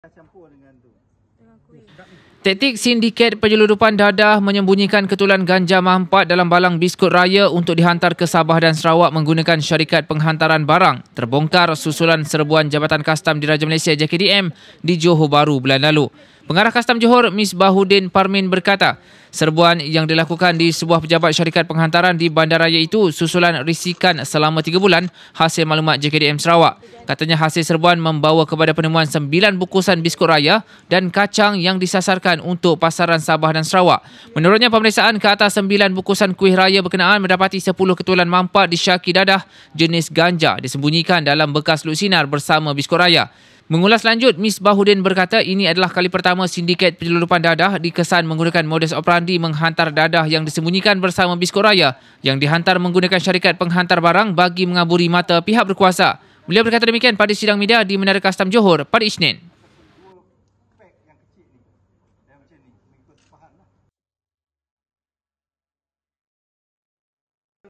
Guna balang biskut raya untuk seludup ganja
JOHOR BAHRU: Taktik sindiket penyeludupan dadah menyembunyikan ketulan ganja mampat di dalam balang biskut raya sebelum menggunakan syarikat penghantaran barang untuk dihantar ke Borneo, terbongkar dalam satu serbuan, bulan lalu.